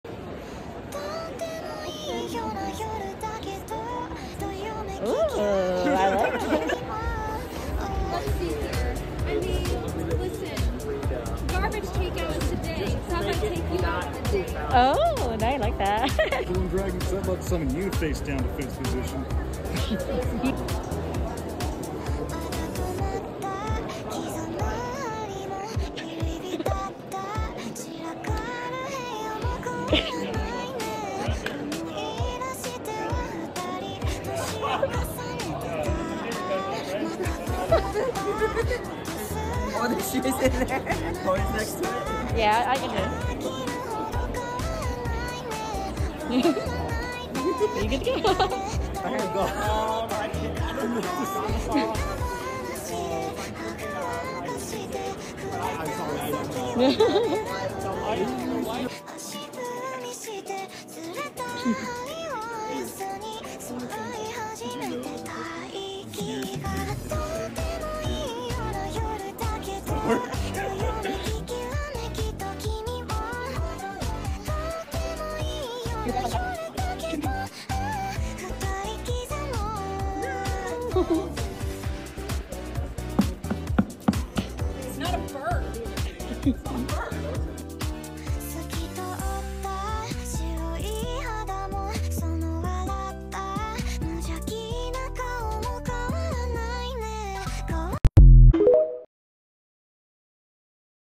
Rizzing up the trash can at Anime Frontier
having fun in Fort Worth Texas as Trailblazer having people rizz up the trashcan. It was a blast! I don’t own the music!!!